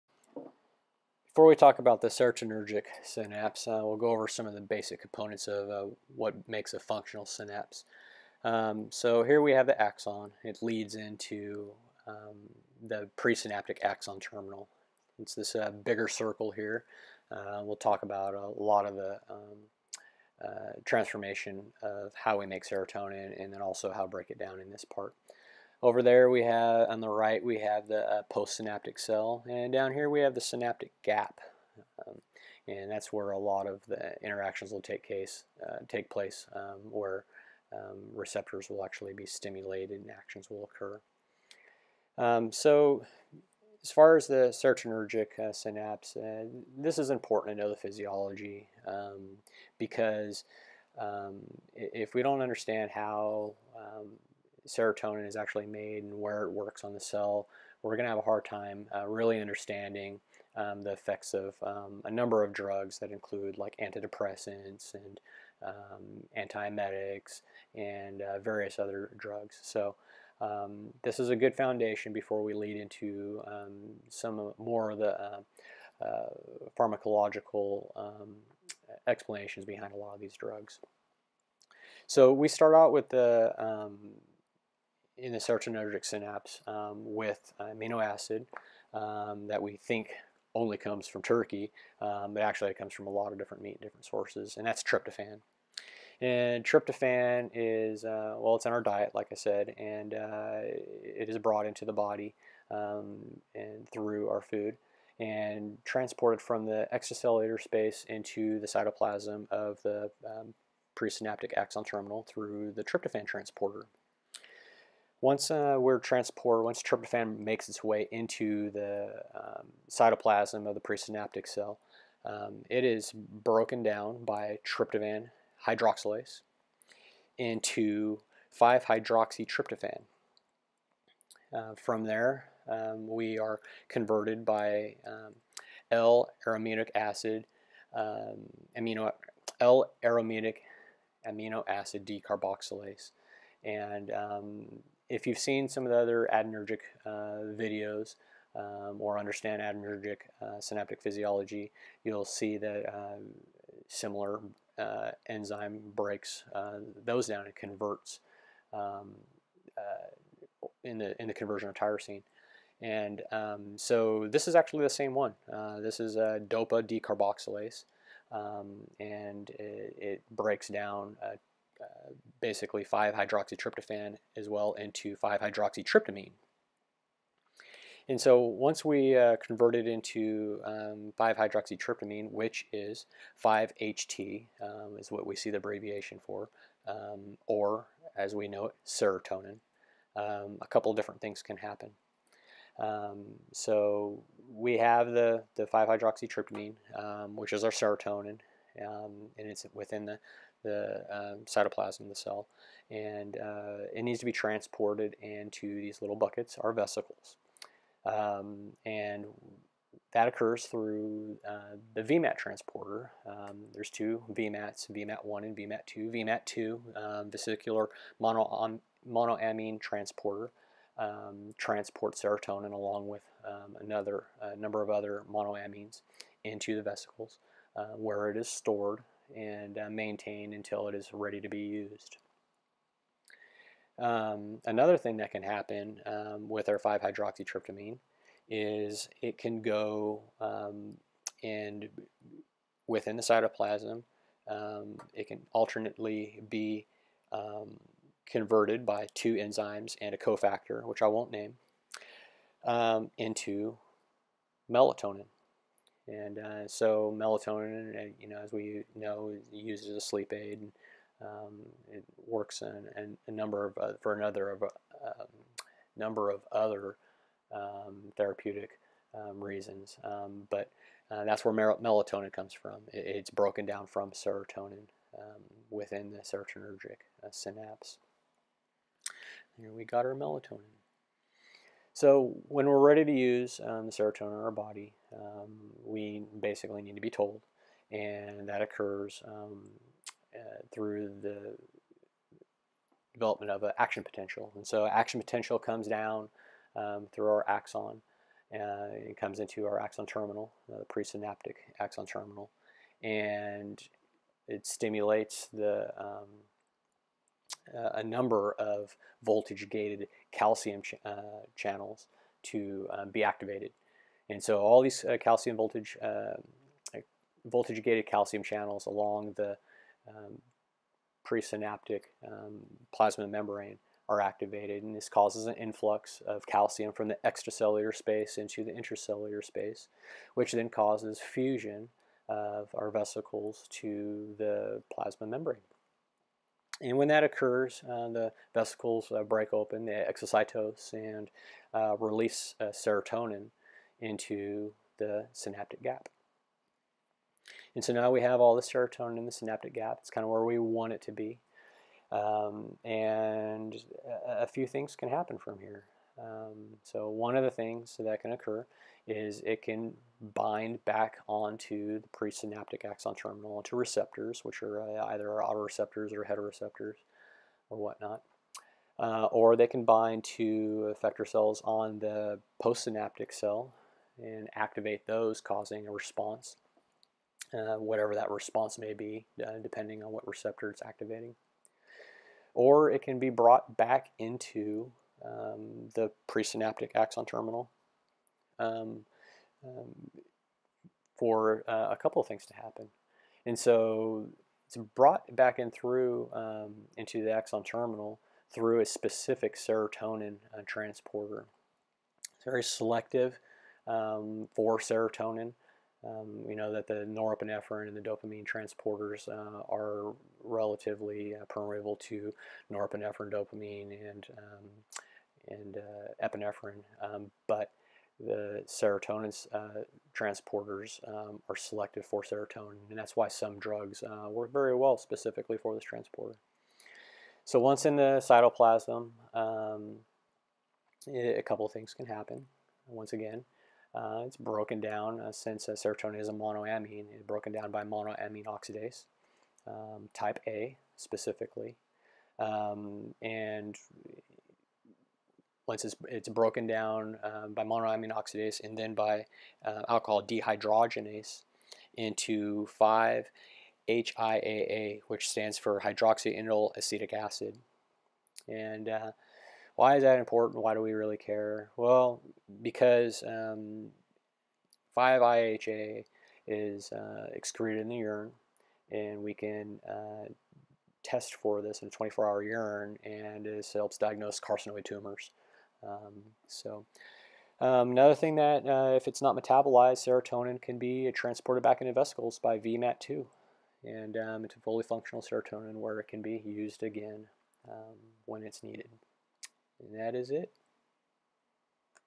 Serotonergic Synapse
A "BASIC" explanation of how a Serotonergic synapse works. This is a starter video mean to build a foundation for Serotonergic receptor theory, Serotonergic pharmacology and how specific Serotonergic medications works within the body.